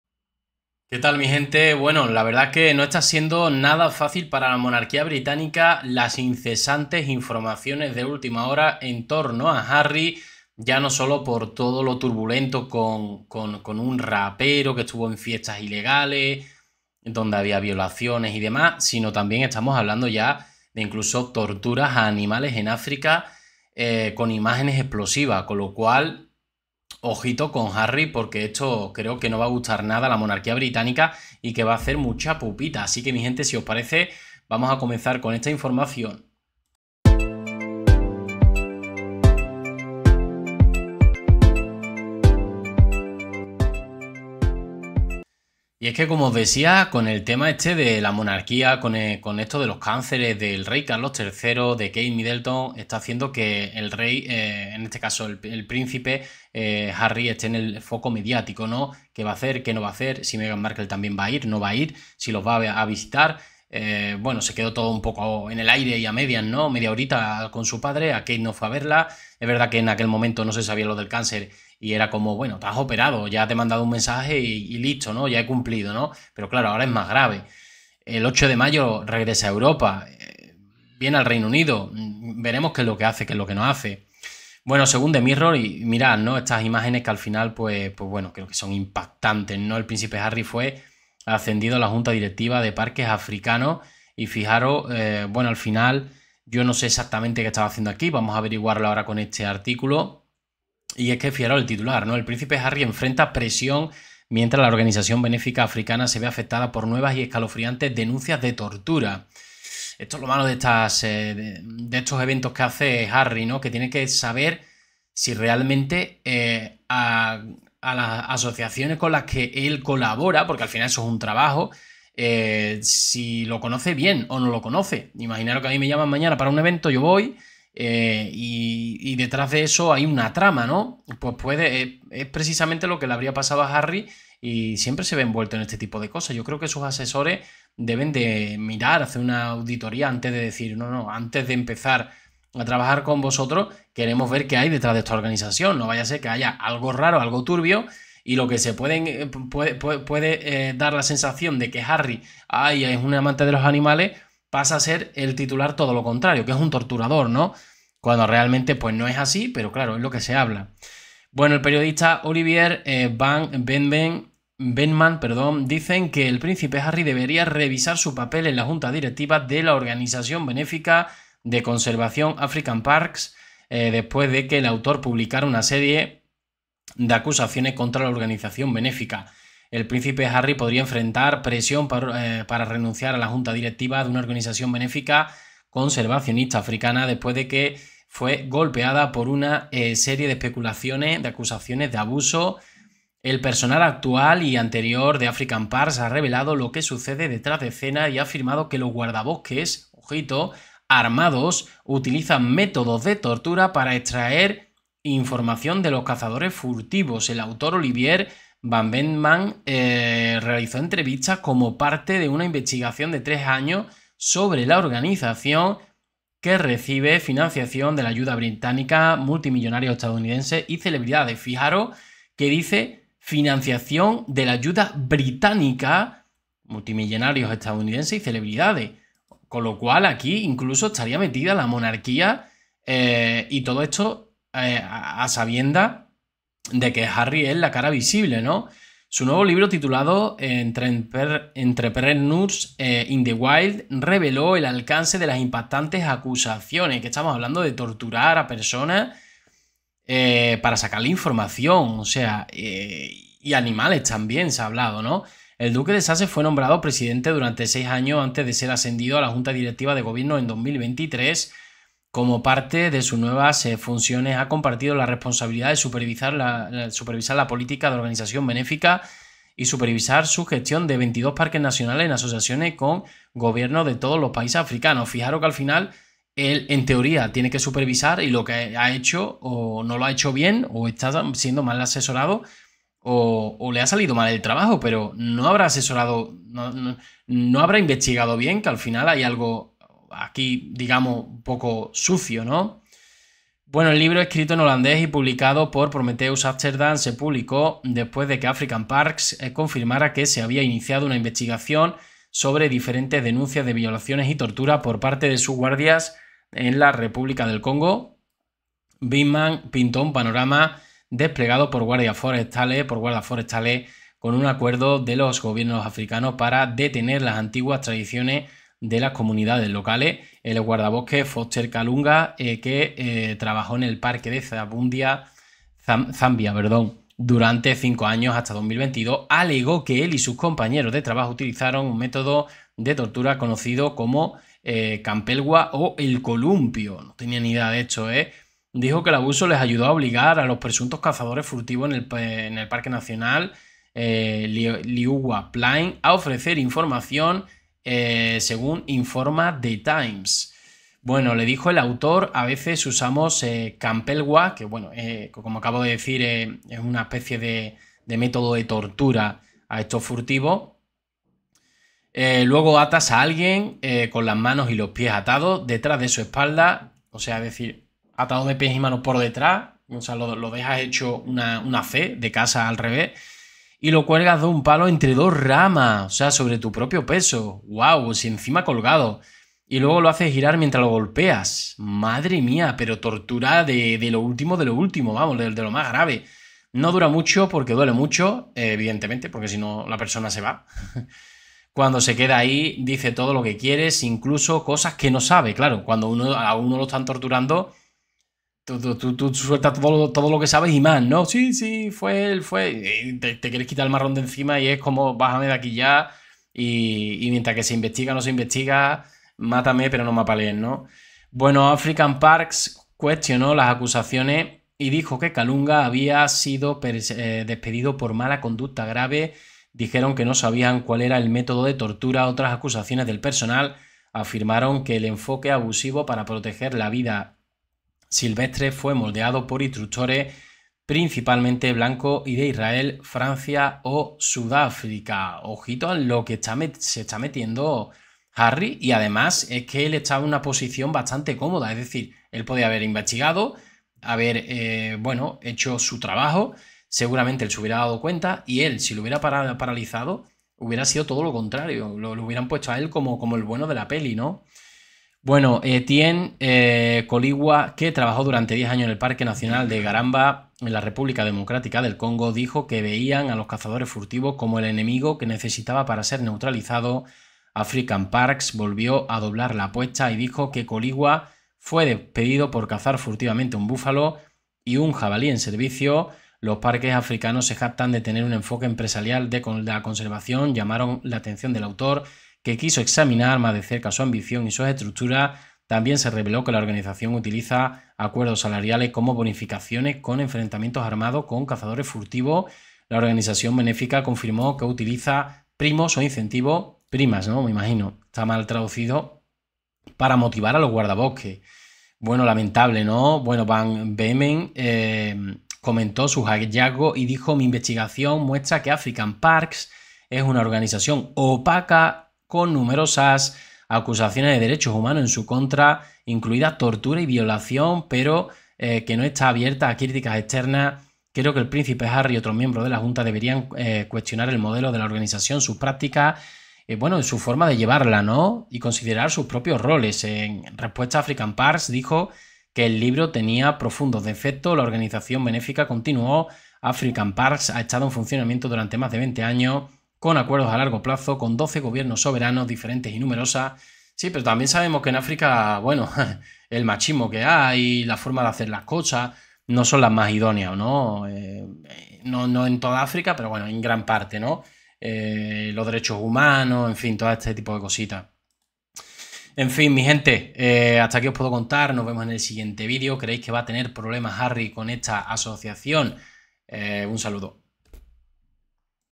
EL PRÍNCIPE HARRY ACUSADO DE TORTURAR Y VI0LAR A FURTIVAS, EN SU ASOCIACIÓN BENÉFICA
X @garcia_youtube
GRUPO WHATSAPP AVISOS DIRECTOS Y VÍDEOS👇